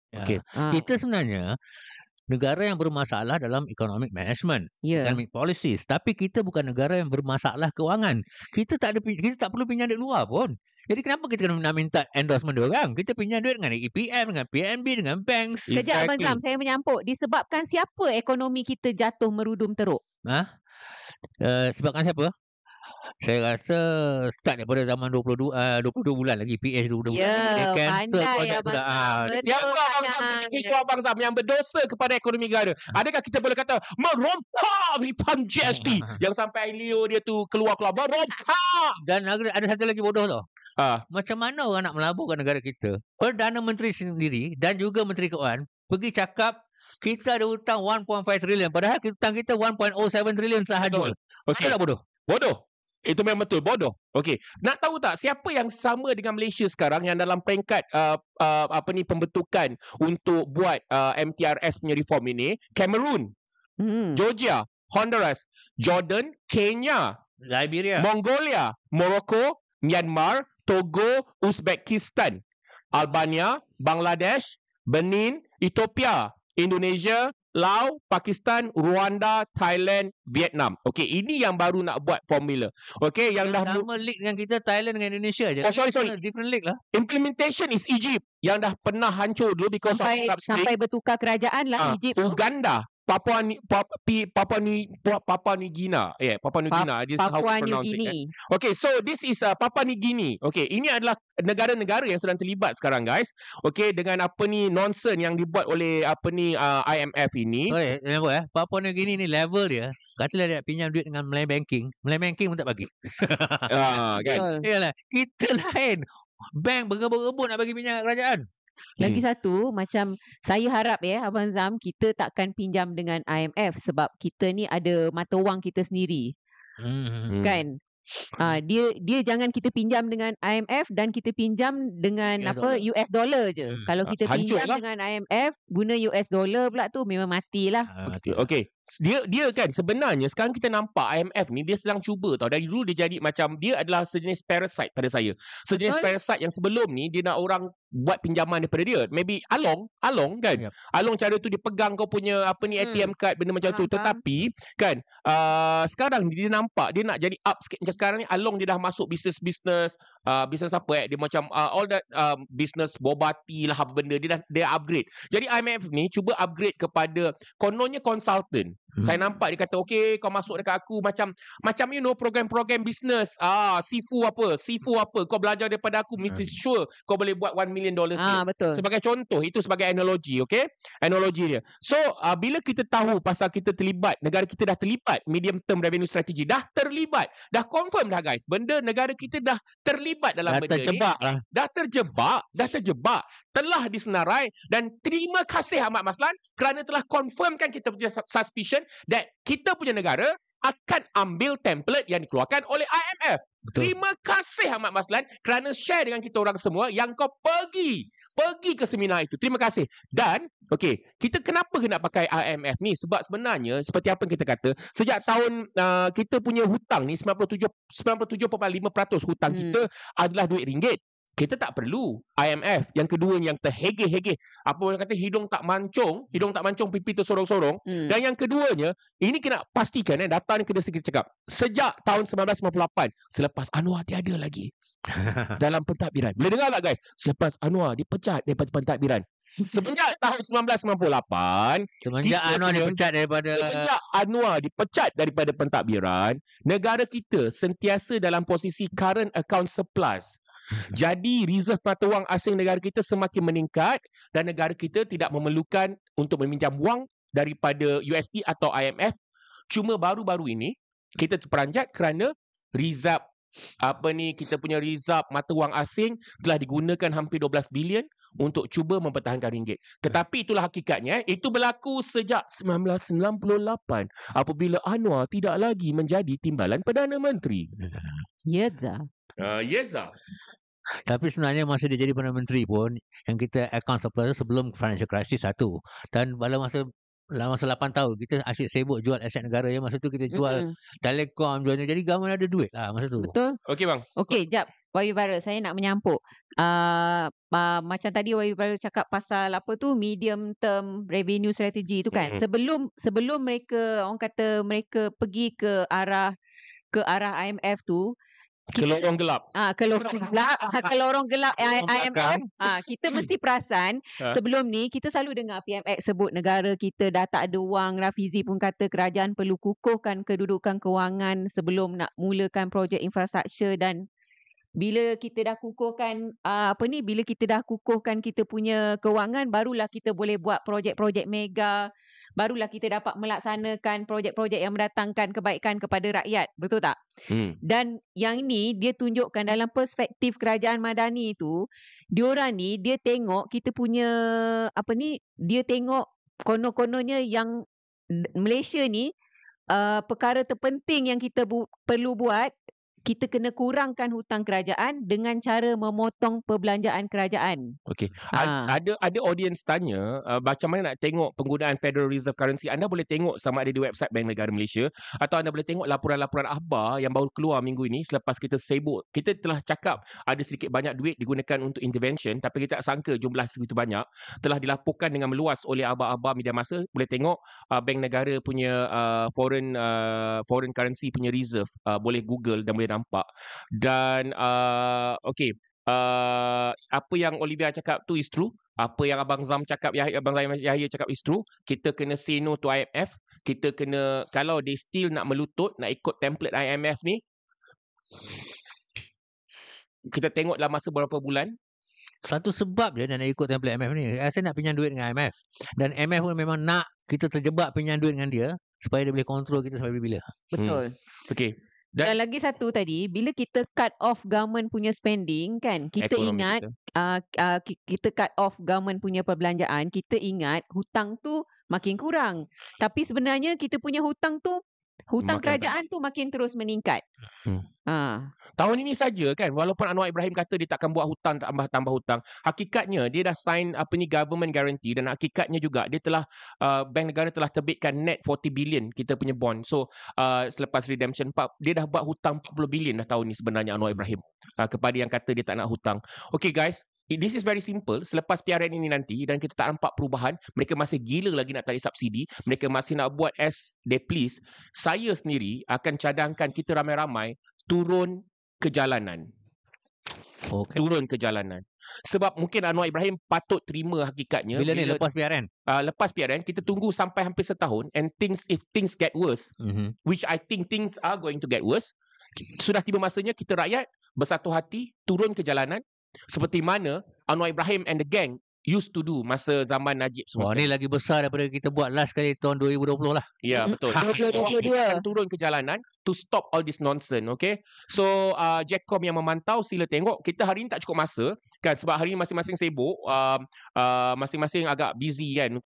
SELAMATKAN MALAYSIA! IMF BAKAL JADI PENJAHANAM NEGARA? (BAHAGIAN 3)
YB Pontian merangkap Menteri Kewangan I Datuk Seri Ahmad Maslan telah mendedahkan bahawa MOF dan beberapa agensi kerajaan lain telah menyertai satu kursus yang dianjurkan oleh IMF (Tabung Kewangan Antarabangsa) di Kuala Lumpur baru-baru ini.  Adakah fahaman dan ideologi ekonomi IMF akan diterapkan ke dalam pentadbiran Kerajaan Anwar-Zahid seiring dengan matlamat PMX untuk menarik balik subsidi dan meningkatkan hasil cukai?